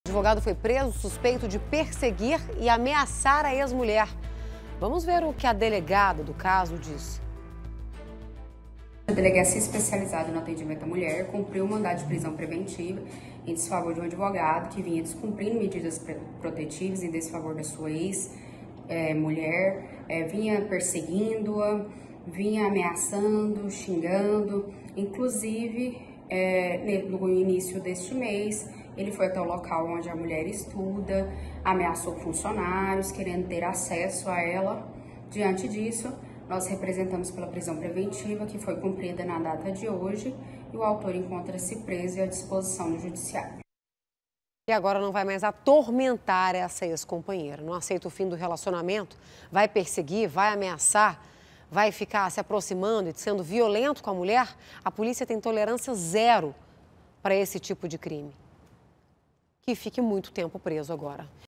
Advogado perseguia e ameaçava a ex-mulher
Advogado é preso suspeito de perseguir e ameaçar a ex-mulher. Vamos ver o que diz a delegada do caso.